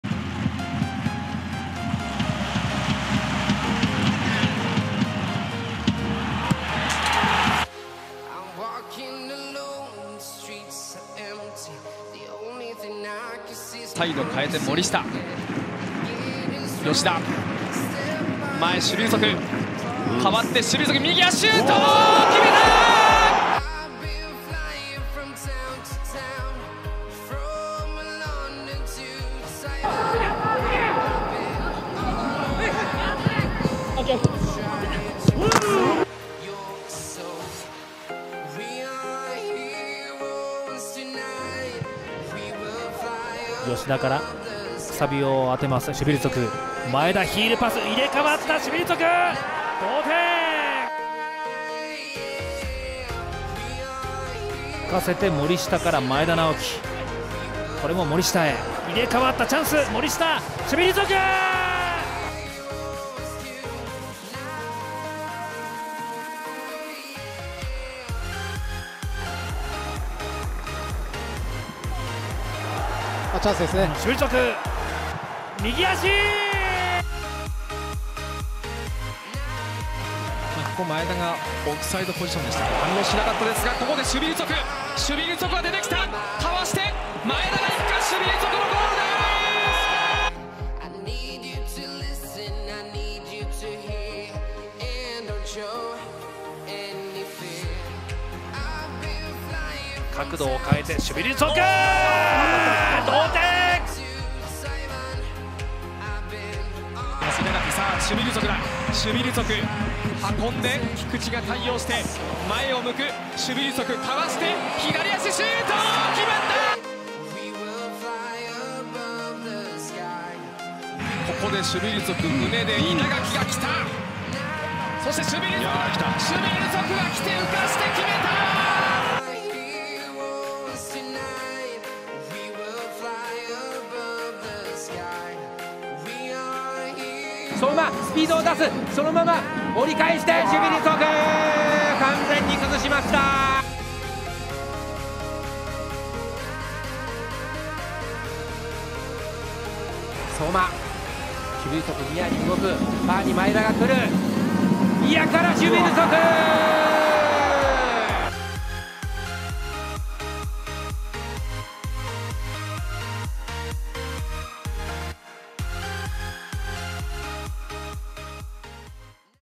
シュヴィルツォク 名古屋グランパス 2021年ゴール集 全12ゴール Jリーグ・ルヴァンカップ・天皇杯・ACL
シュヴィルツォク　名古屋グランパス　2021年ゴール集

今回はシュヴィルツォク選手の2021年シーズン全ゴールをまとめました。